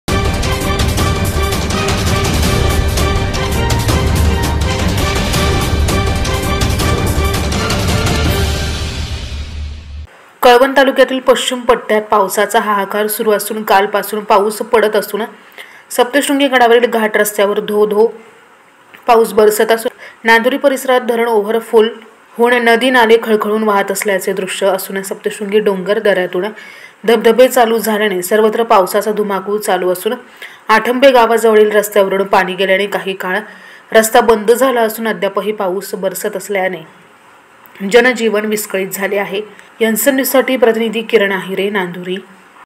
नांदुरी । कळवण तालुक्यातील पश्चिम पट्ट्यात पावसाचा हाहाकार
NCN News